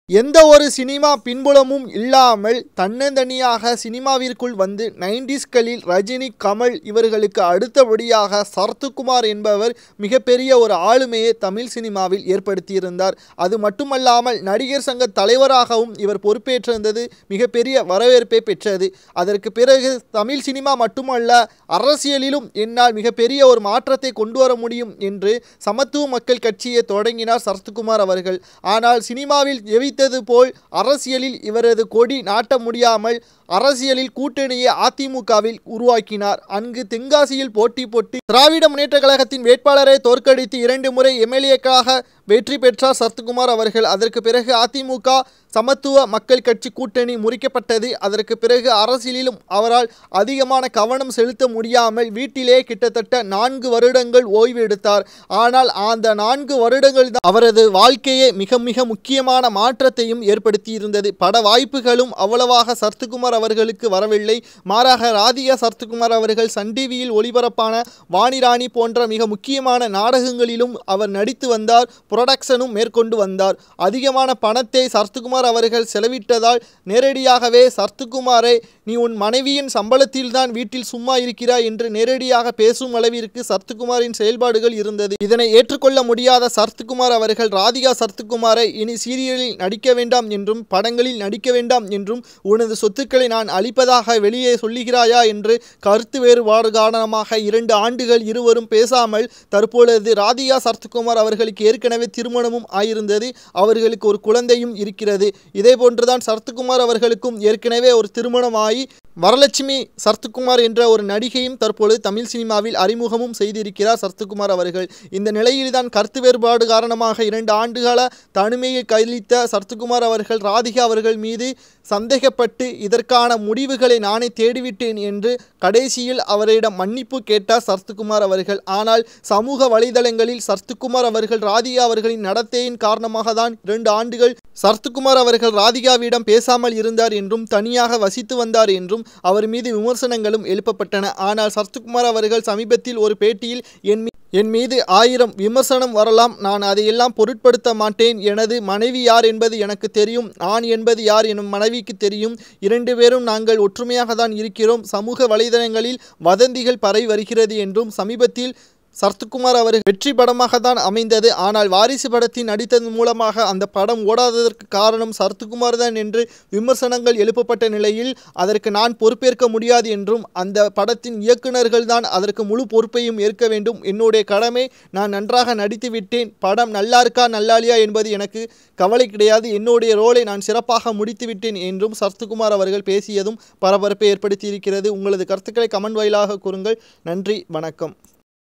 😱ராதிகாவின் தப்பான உறவால் ..! சரத்குமார் எடுத்த திடீர் விபரீத முடிவு..! அதிர்ச்சியில் திரையுலகம்!
😱 கடைசி ஆசையே நிறைவேற்றிய விஜய் ஆண்டனி..! மீரா கல்லறையில் நடந்த நேரலை..!